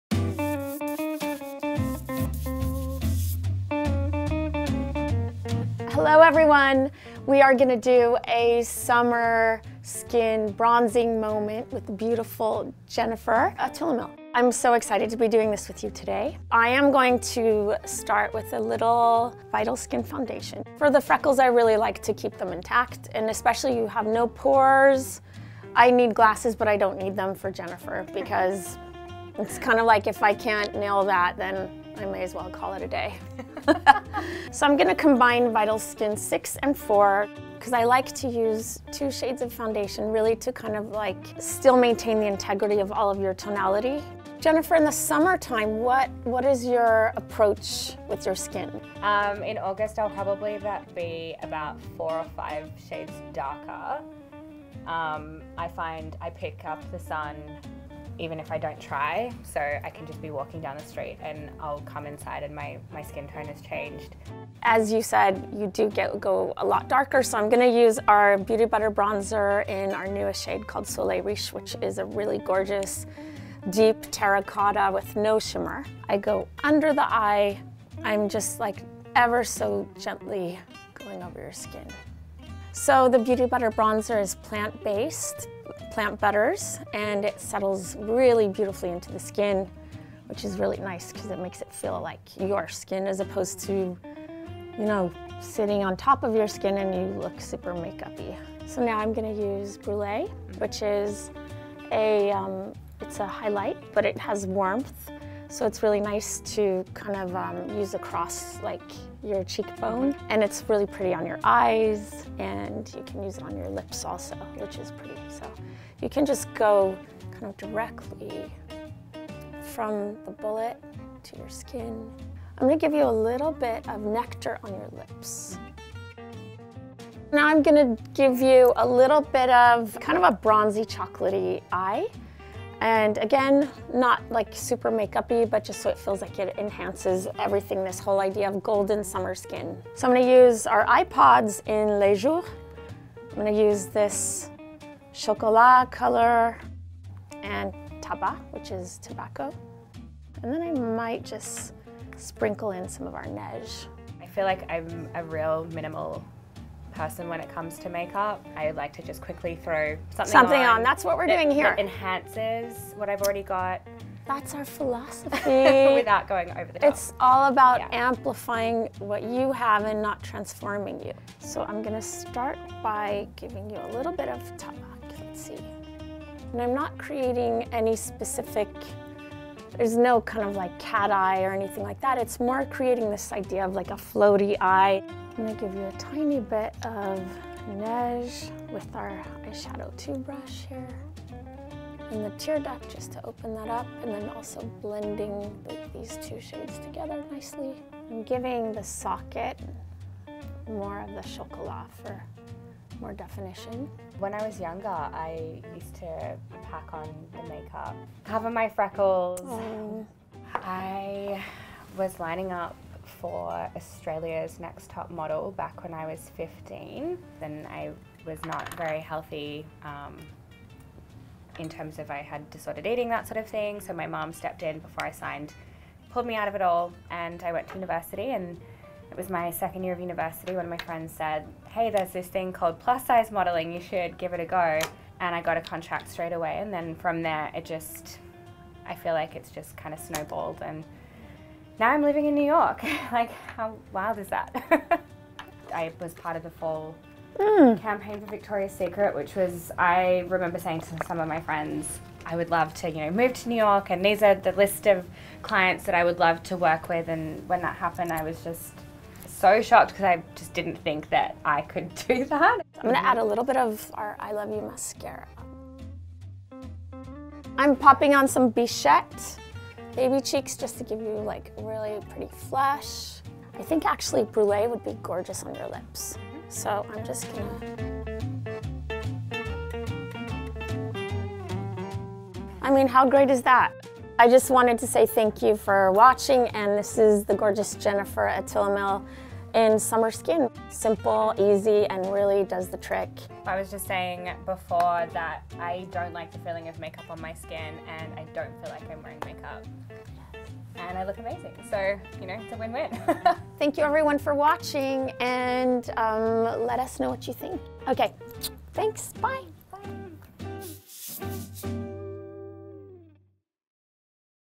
The "Summer Skin" Masterclass for a Golden Bronze Glow
Makeup artist Gucci Westman amps up model Jennifer Atilémile's skin with a golden-bronze look that's sun-kissed, striking, and believably natural. Plus, how the rising star got her big break—and what it feels like to bring body inclusivity to Victoria's Secret.

Discover Gucci's Guide, our blog with more makeup how-tos, beauty adventures, and black book listings here!